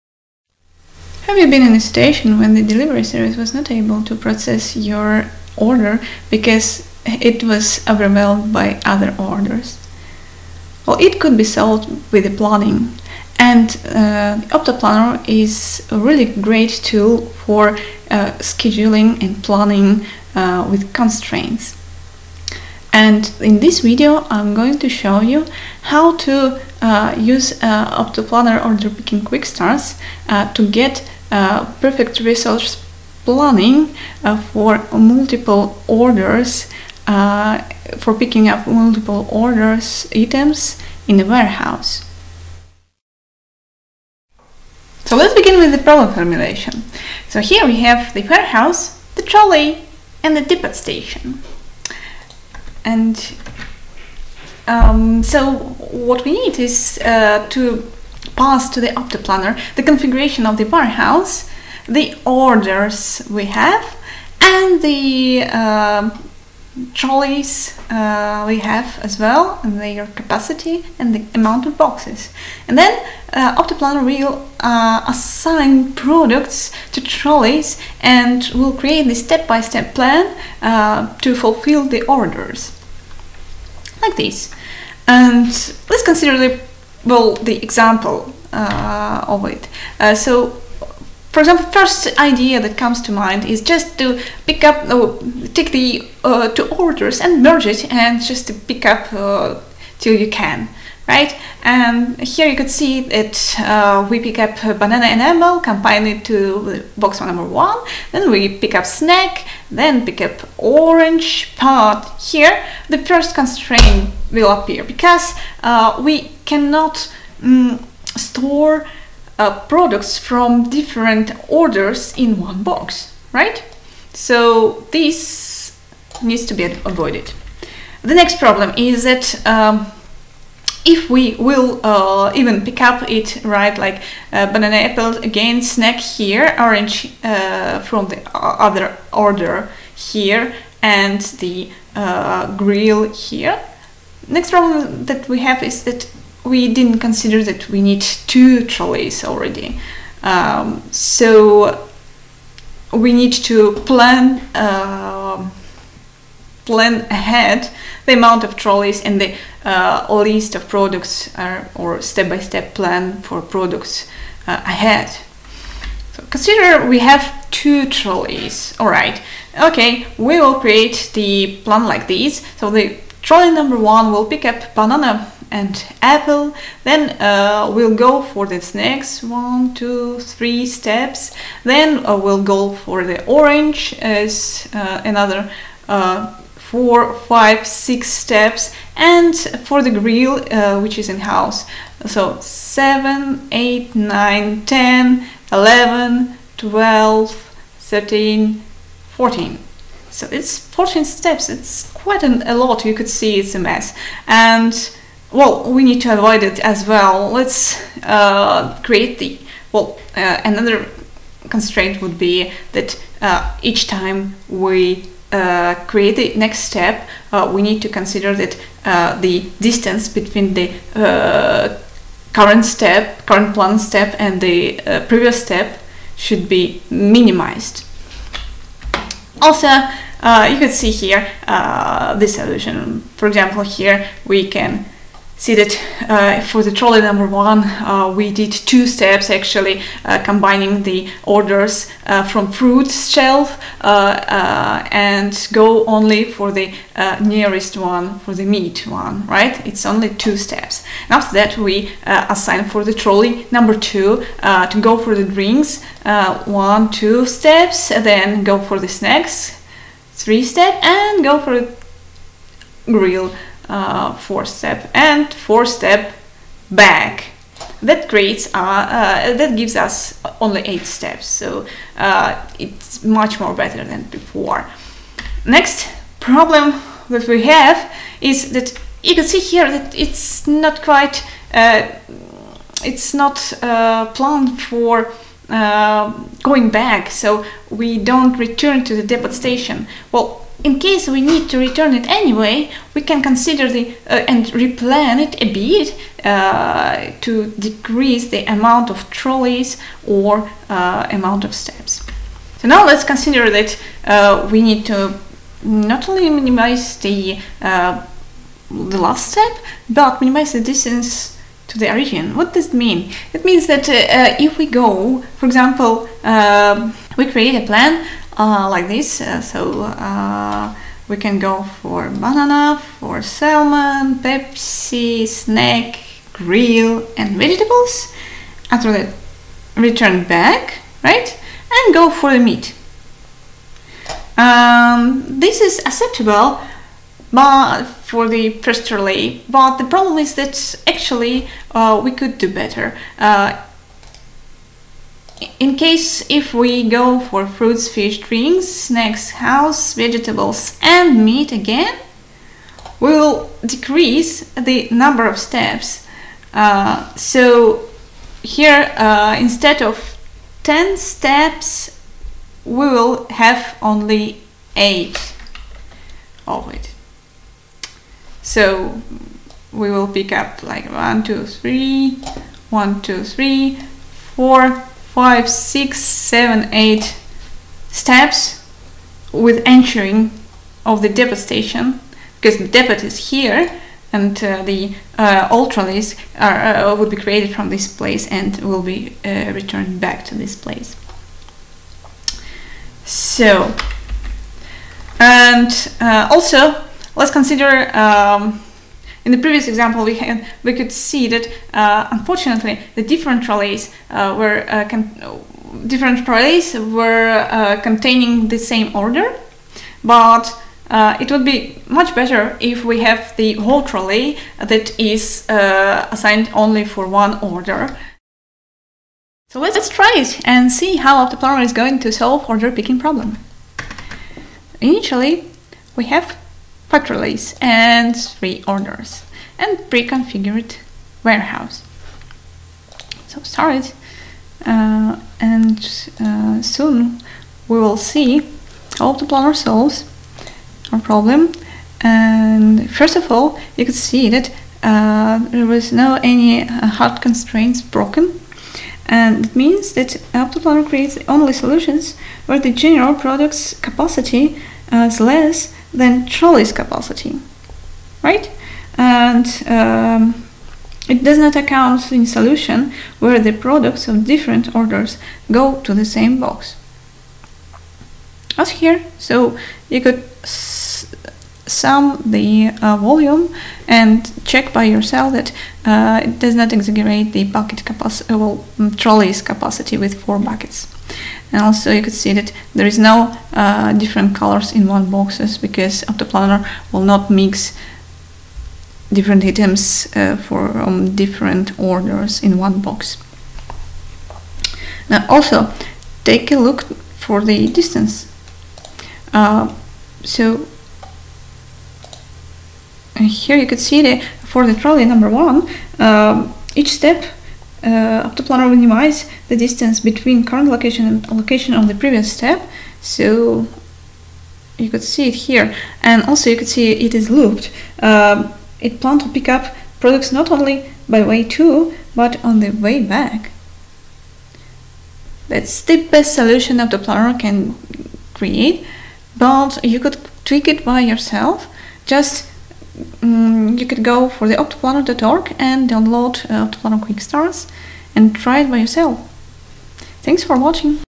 Order picking planning with OptaPlanner
How to solve order picking problem?
Generate an optimal picking plan for completing a set of orders.

This video shows how to use OptaPlanner Order picking quick start to assign products to trolleys, if you need to pick up multiple orders simultaneously and minimize the distance covered by each trolley. It demonstrates the meaning of 4 constraints and how do they make the solution optimal.

00:00 Introduction
00:44 Problem formulation
01:30 Order picking constraints
7:00 Demo OptaPlanner order-picking